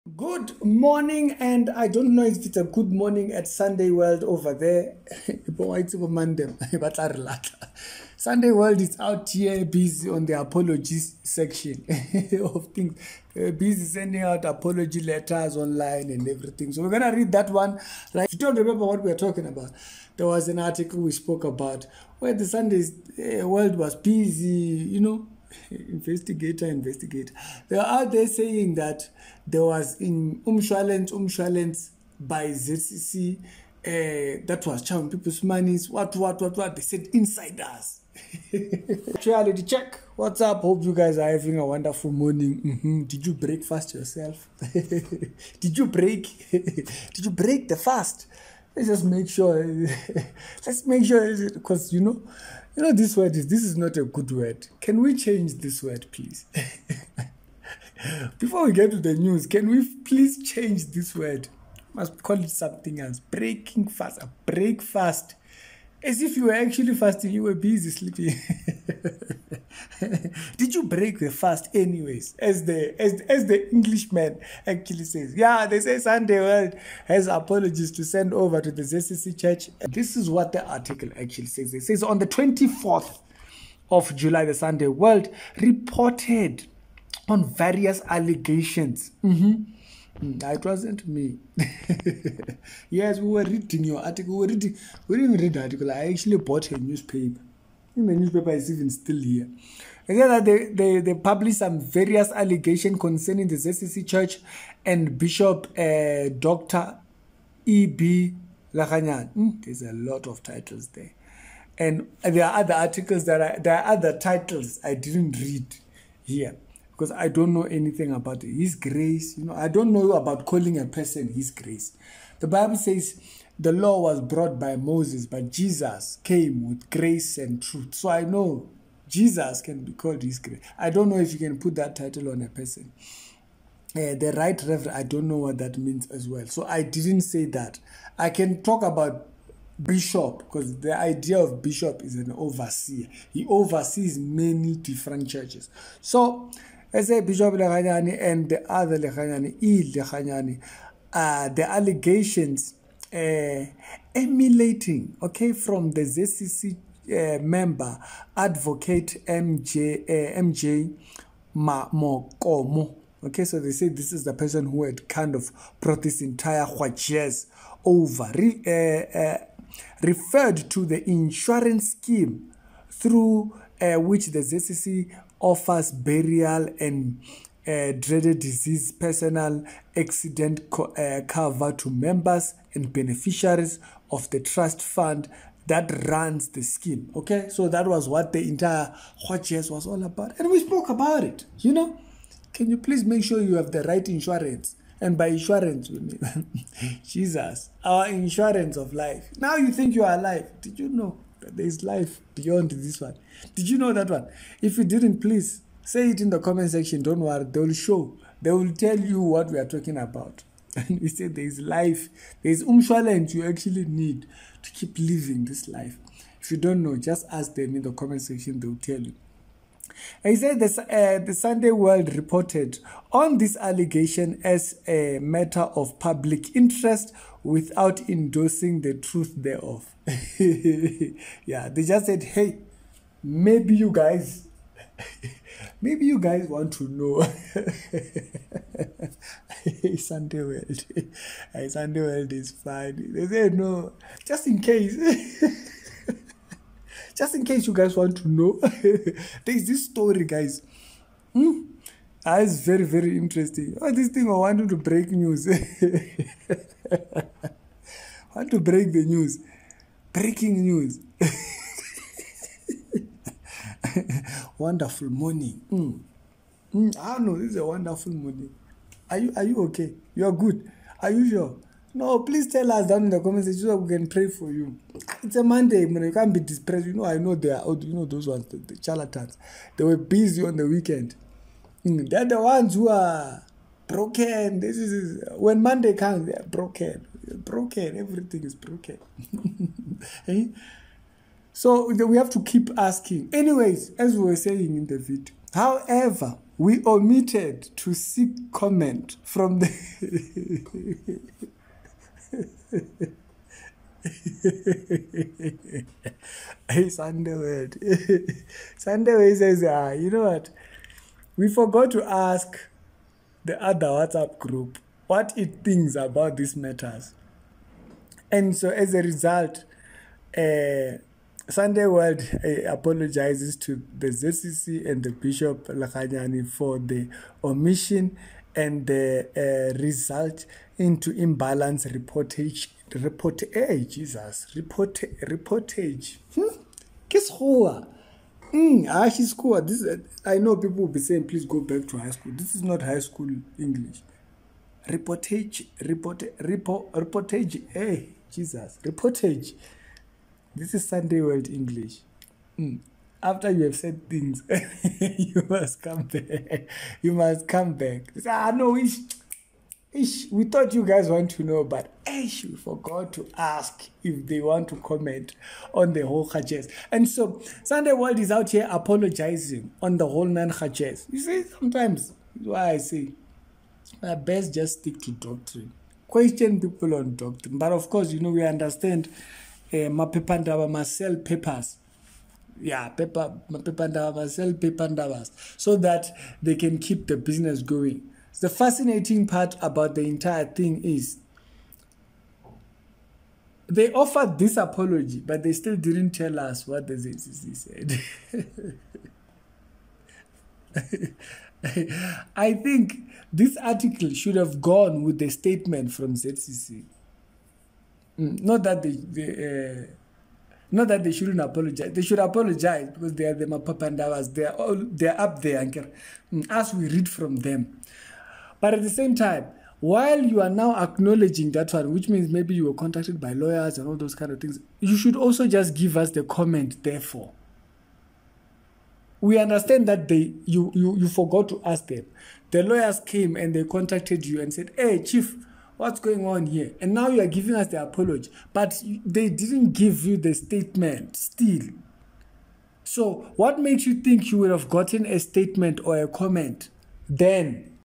Update: ZCC Apology from Sunday World
ZCC Apology from Sunday World after insurance allegations article.